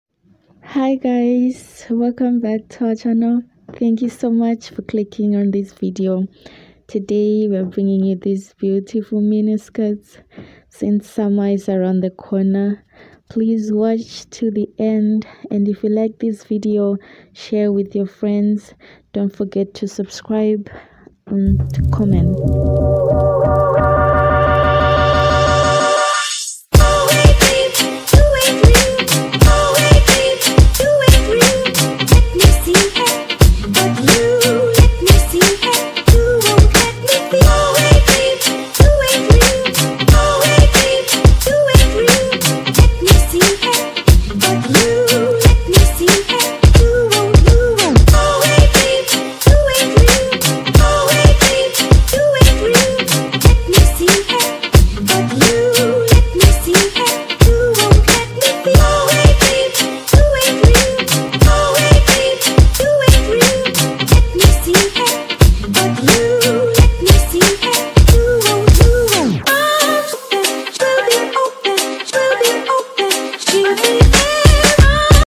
Curvy mini skirts//summer skirts 2021
Today we are bringing you mini skirts for curvy women all around the world